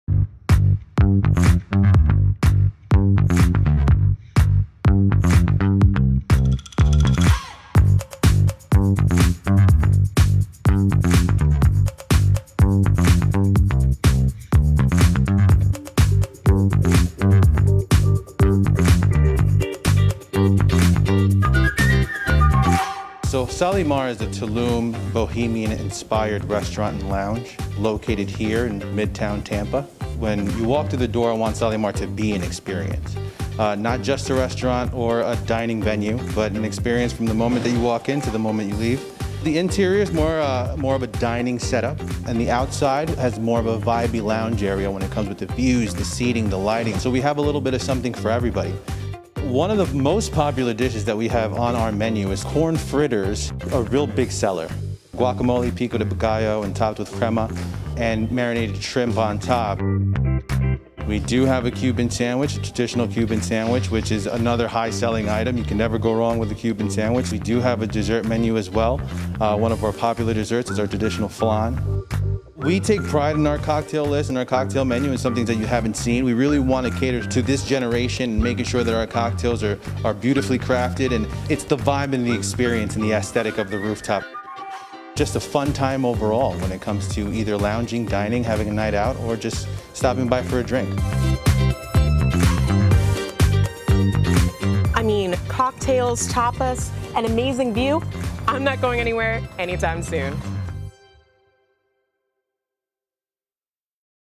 Sal Y Mar Rooftop Bar is open in Midtown Tampa | Taste and See Tampa Bay
Another rooftop bar has been added to Tampa Bay's impressive list. Sal Y Mar, located on top of the Aloft and Element hotels, is one of the first restaurants to open in Midtown Tampa.